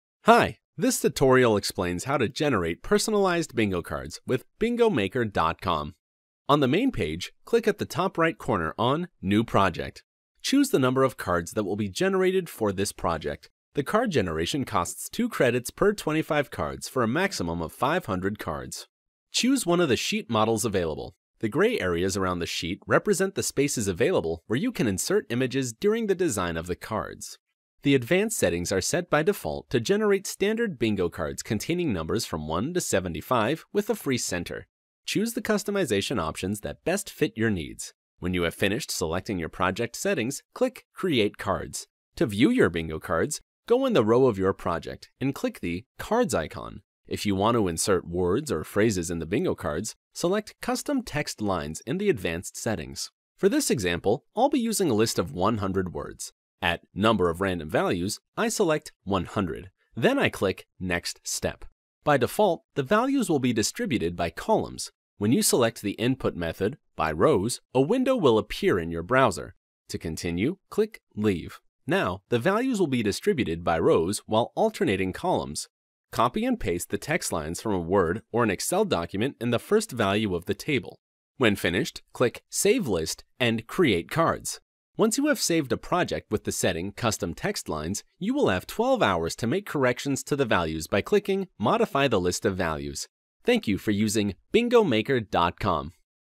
How to make custom bingo cards with Bingo Maker
Script:
Hi, this tutorial explains how to generate personalized bingo cards with Bingo Maker dot com.
On the main page, click at the top-right corner on “New Project“.
Choose the number of cards that will be generated for this project. The card generation costs 2 credits per 25 cards for a maximum of 500 cards.
Choose one of the sheet models available.
The gray areas around the sheet represent the spaces available where you can insert images during the design of the cards.
The advanced settings are set by default to generate standard bingo cards containing numbers from 1 to 75 with a free center. Choose the customization options that best fit your needs.
When you have finished selecting your project settings, click “Create cards“.
To view your bingo cards, go in the row of your project and click the "Cards” icon.
The last project created or edited will be shown at the top left hand corner of the main page.
If you want to insert words or phrases in the bingo cards, select “Custom text lines” in the advanced settings.
For this example, I’ll be using a list of 100 words. At “Number of random values” I select “100”.
Then I click “Next step“.
By default, the values will be distributed by columns.
When you select the input method “By rows“, a window will appear in your browser. To continue, click “Leave“.
Now, the values will be distributed by rows while alternating columns.
“Copy and paste” the text lines from a Word or an Excel document in the first value of the table.
When finished, click “Save list and create cards“.
Once you have saved a project with the setting “Custom text lines”, you will have 12 hours to make corrections to the values by clicking “Modify the list of values“.
Thank you for using Bingo Maker dot com.